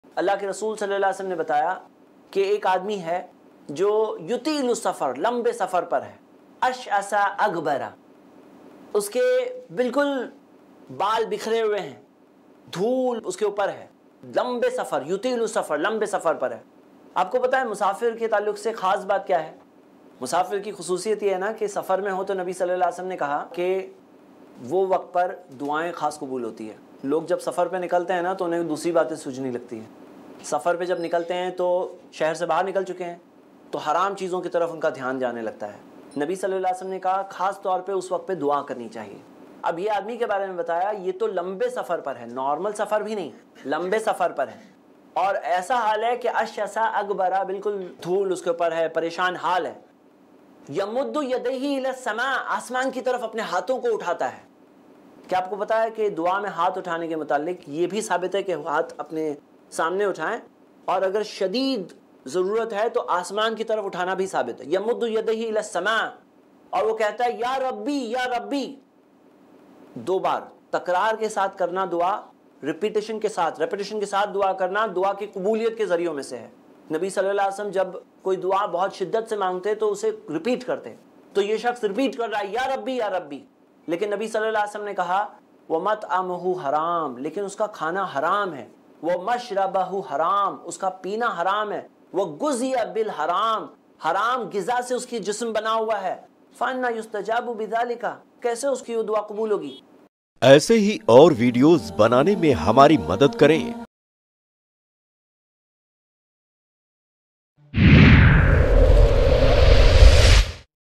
Aise Insaan Ki Dua Qubool Nahi Hot Kyunki.... | Zaid Patel iPlus TV
Is Video me aap dekh rahe hain:
Halal Rozi kyu zaroori hai? Aur Haram Rozi ka nuqsan kya hoga?
Haram Rozi khane wale ko ek bada Nuqsan uski Dua ka Qabool na hona hai.
Uski Dua Qabool nahi hoti. Log kabhi kabhi shikayat karte hain ki Dua Qabool kaise hogi ya Hamari Dua Qabool kyu nahi hoti? aise log dekhe kahi Haram khane ka yeh nuqsan to woh nahi jhel rahe hain.
Keep watching full video ...